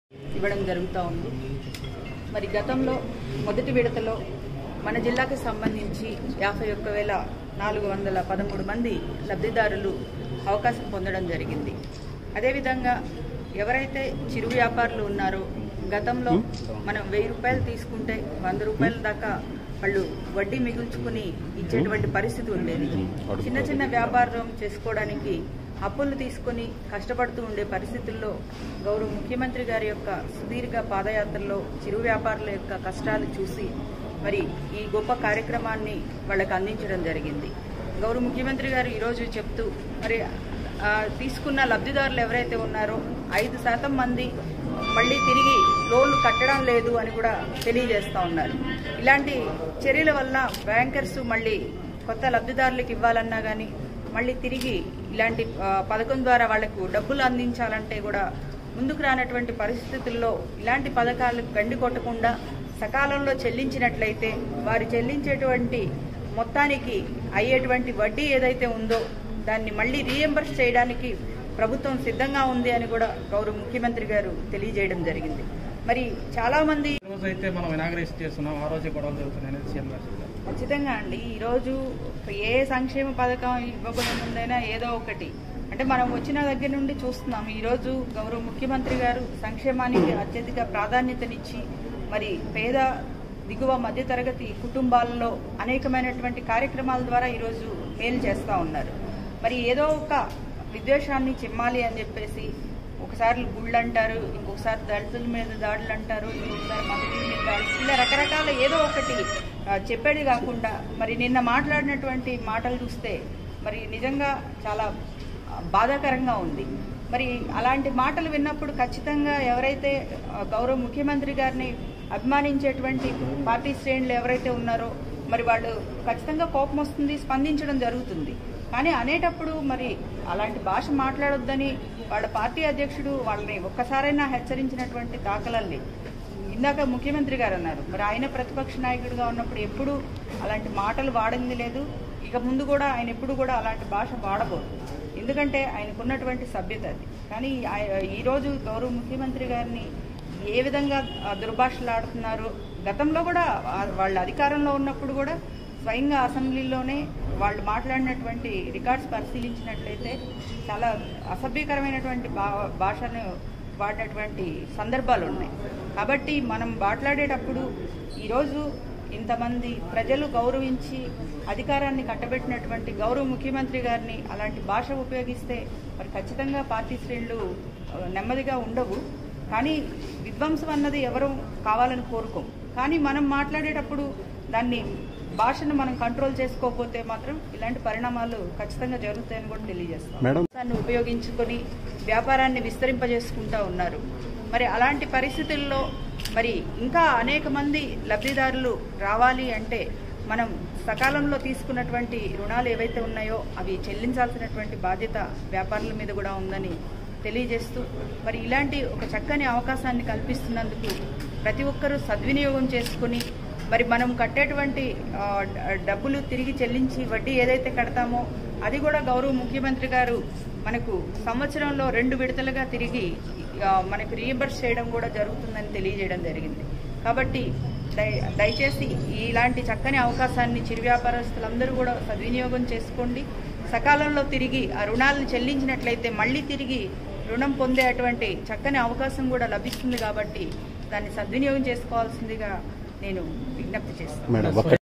AP Home Minister Mekathoti Sucharita on disbursing Jagananna Thodu welfare scheme || Guntur
AP Home Minister Mekathoti Sucharita on disbursing Jagananna Thodu welfare scheme || Guntur - 20th October 2021